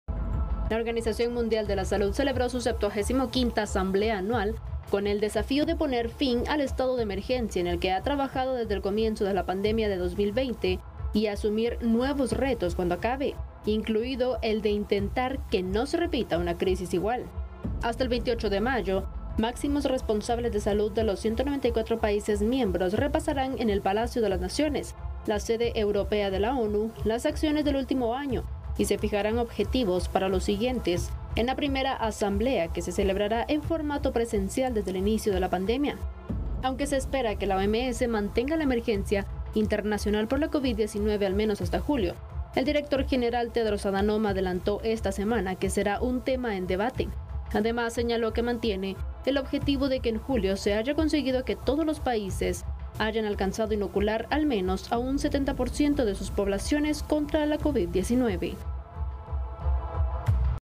¿Se terminaría el estado de emergencia por Covid 19?
La OMS podría poner fin al estado de emergencia por covid en julio

La Organización Mundial de la Salud (OMS) celebra este domingo su 75ª asamblea anual con el desafío de poner fin al "estado de emergencia" en el que ha trabajado desde el comienzo de la pandemia de 2020 y asumir nuevos retos cuando acabe, incluido el de intentar que no se repita una crisis igual.